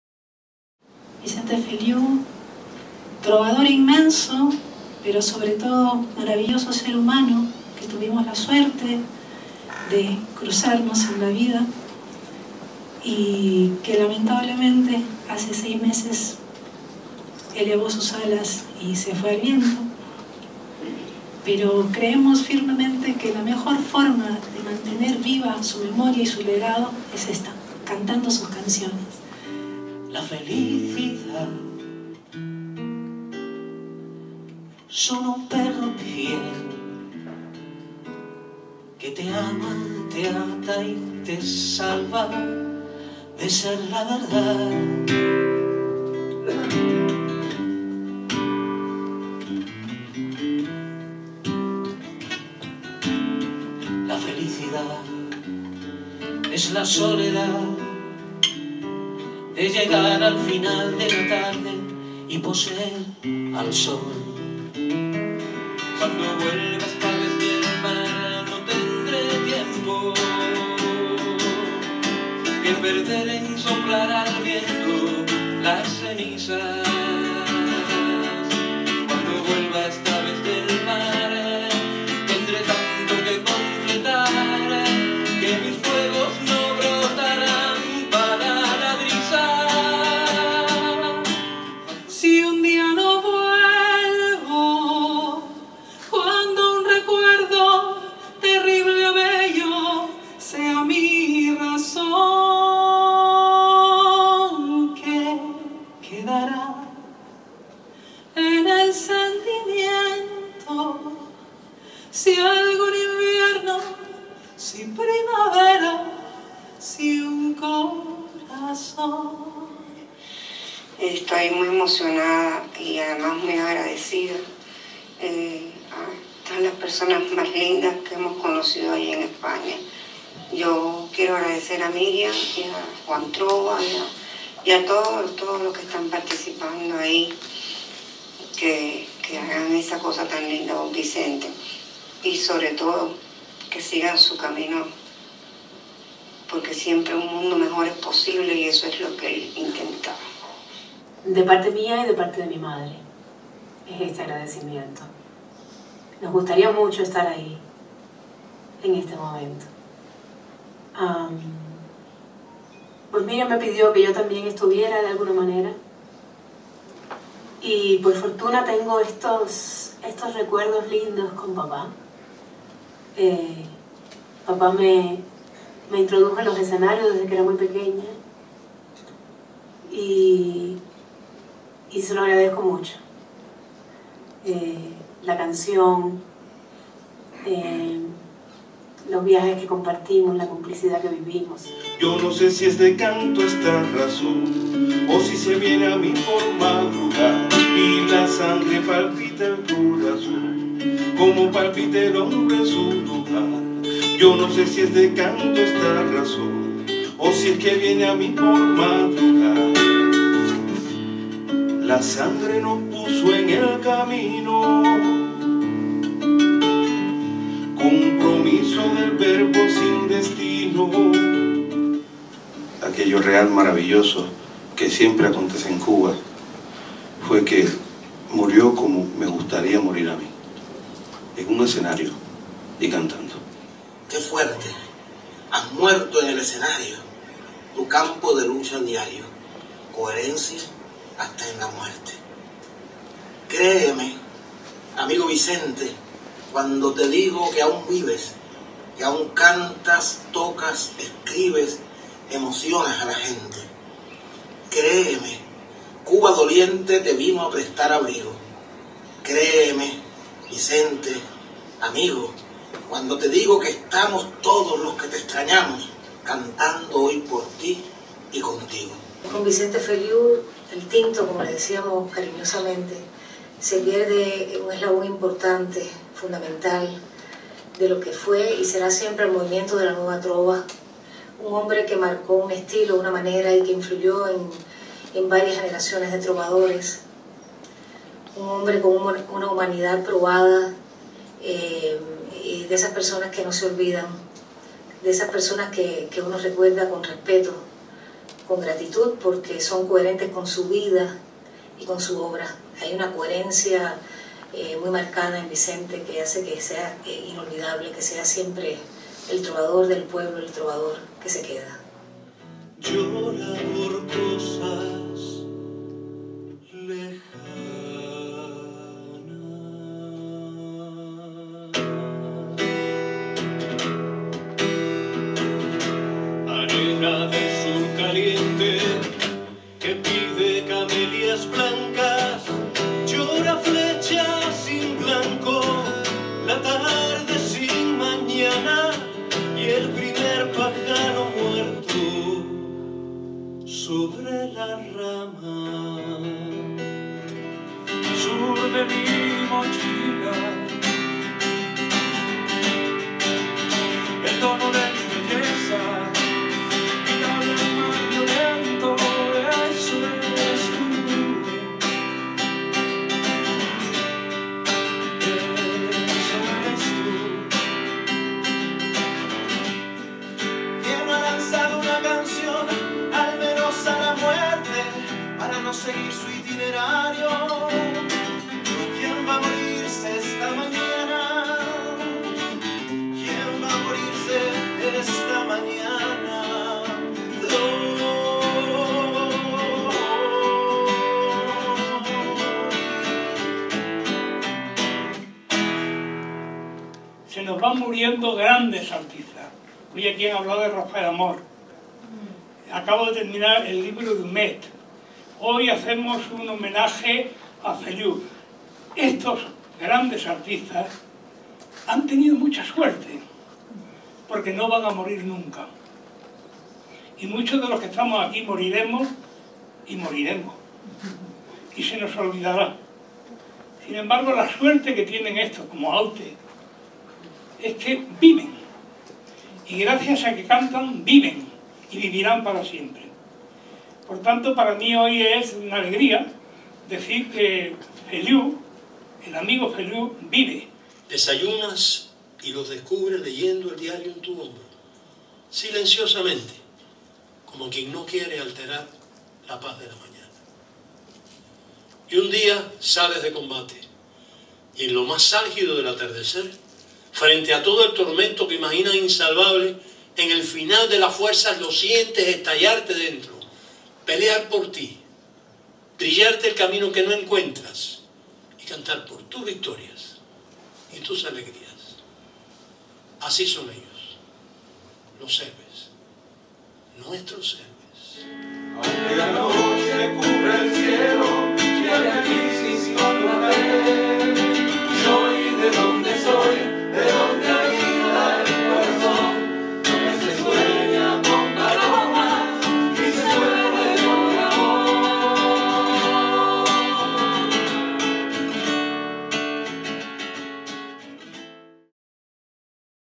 Vicente Feliú vive: así fue el homenaje "Donde habita el corazón" en Madrid (+Fotos)
Cubainformación - Video y fotos: José Luis García de Mingo y Javier García Proenza.- El pasado 20 de junio, bajo el título "Donde habita el corazón: recordando a Vicente Feliú", en la Sala Manuel de Falla de la SGAE de Madrid, se realizó un nuevo homenaje a uno de los más grandes representantes de la Nueva Trova Cubana, Vicente Feliú, fallecido hace seis meses.